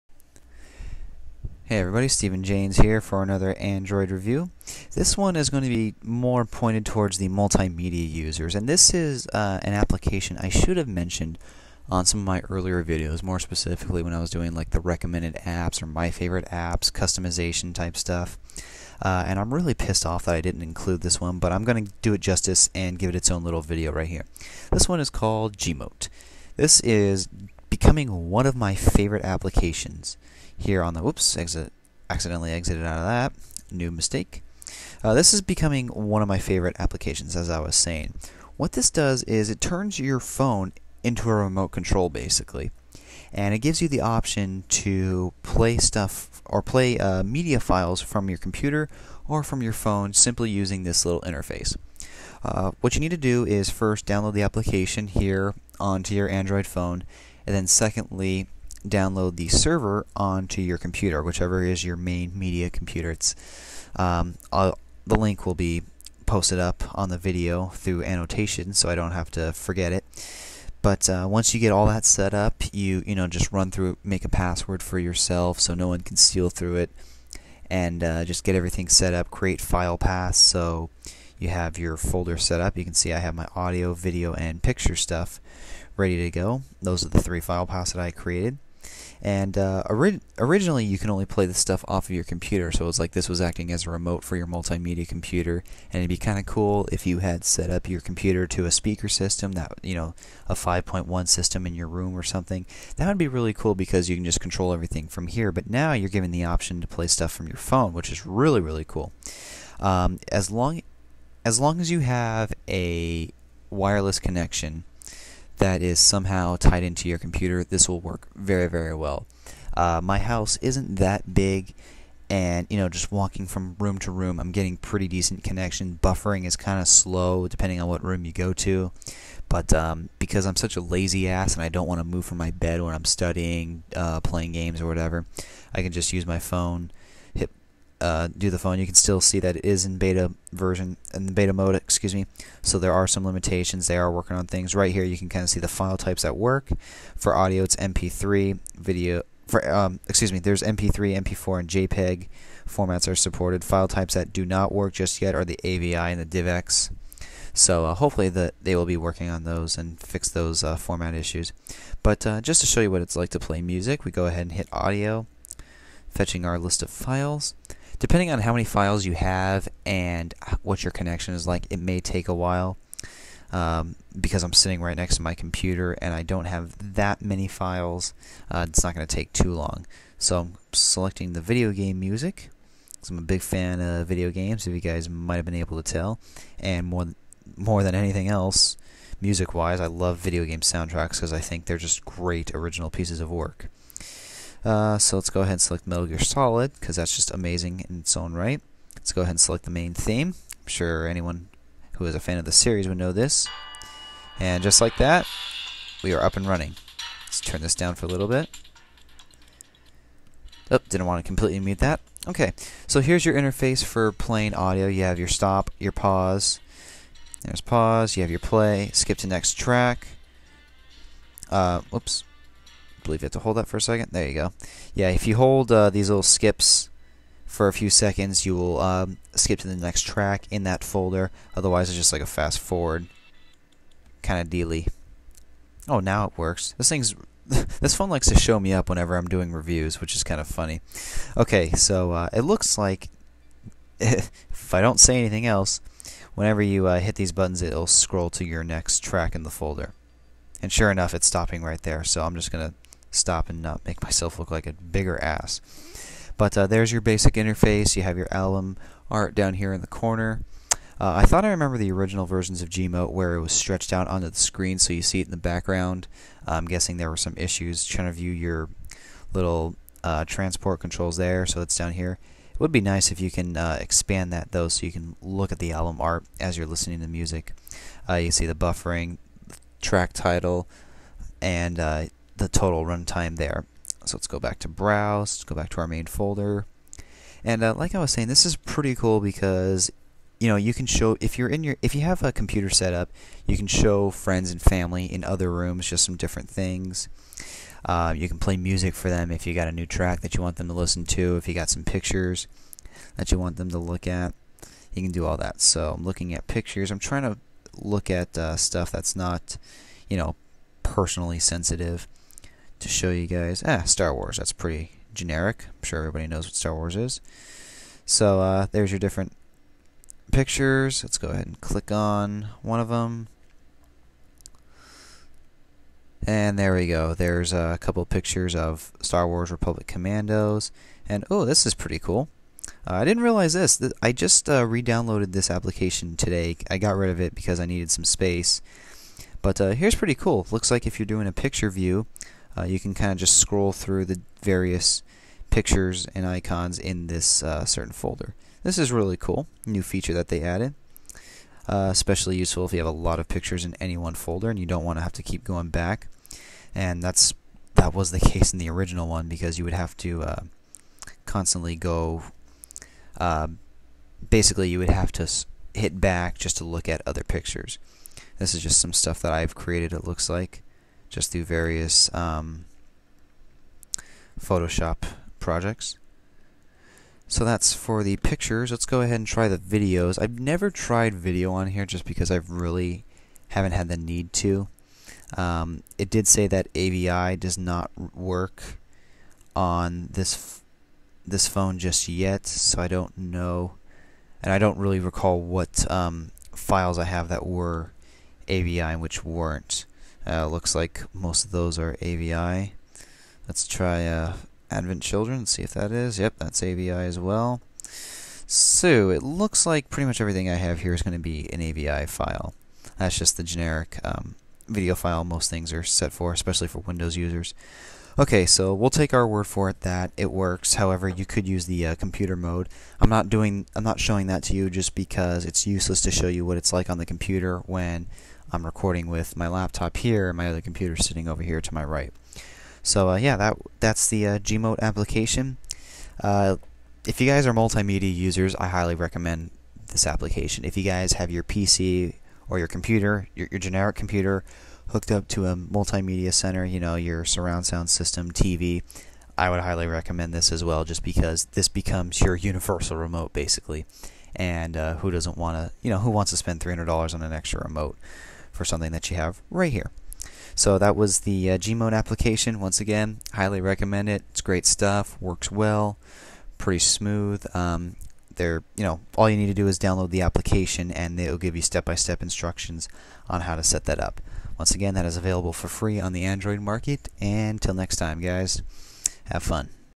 Android Show and Tell: GMote
Gmote 2.0, developed by Marc Stogaitis and Mimi Sun, available for FREE on the Android Market.  Average consumer rating: 4.5/5 stars.

I love this app!  It turns your phone into a remote control for your media computer, basically.  Especially useful when your computer is set-up to your entertainment unit and you don't want to switch to the mouse/keyboard combo.  This thing is awesome and I really suggest it for multimedia users.